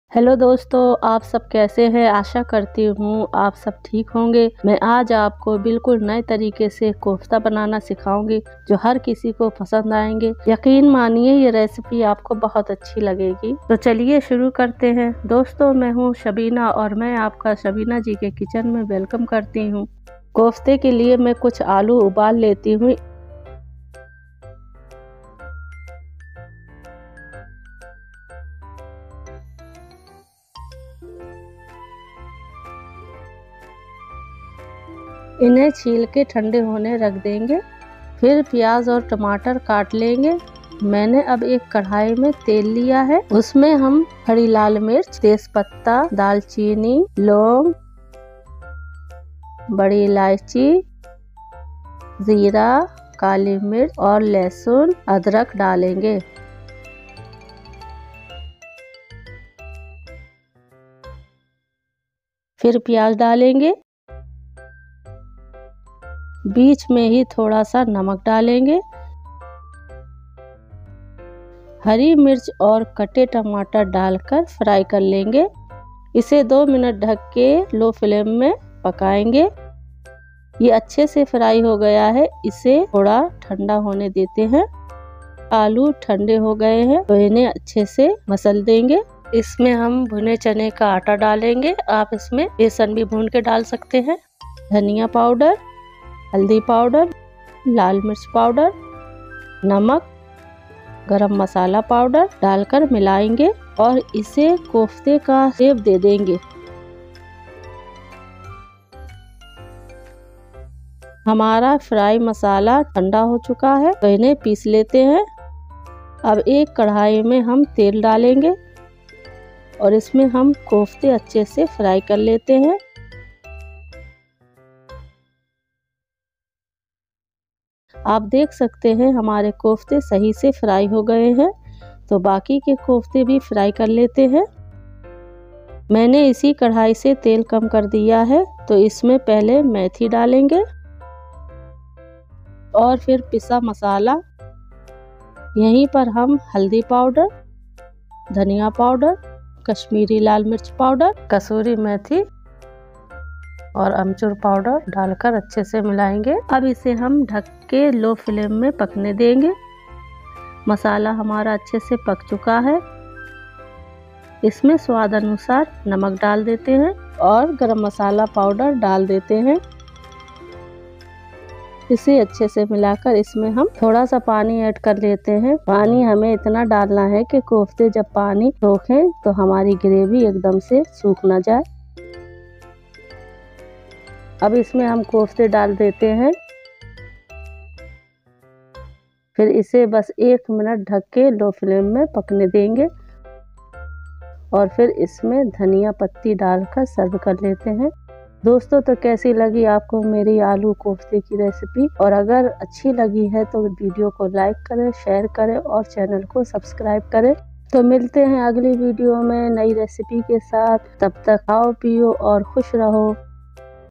Kofta Recipe || ना लोकी ना मीट इस चीज़ से बनाएं स्वादिष्ट कोफते || Kofta Curry Recipe
ना लोकी ना मीट इस चीज़ से बनाएं सबसे स्वादिष्ट कोफते जो सभी चट कर जाएं | Kofta Curry Recipe

Hello Friends Welcome to Shabina Ji Ka Kitchen